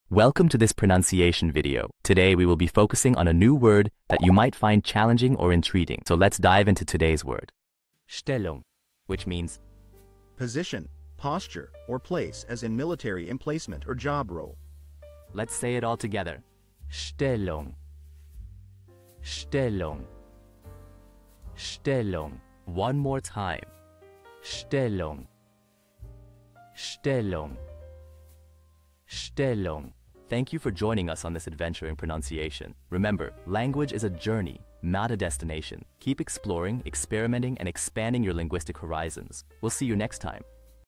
How to pronounce Stellung
Master the Pronunciation of 'Stellung which means position' - which means : Position, posture, or place as in military emplacement or job role. 📝 with @PronunciationGerman 🎓🗣️ - [Your Guide to German]

Learn to pronounce 'Stellung' perfectly in German with our easy and engaging tutorial by @PronunciationGerman! 📖📽️ In this comprehensive video, we provide you with native speaker examples 🗣️👂 and phonetic breakdowns 🎵 to ensure you get the pronunciation just right.

How to say position in German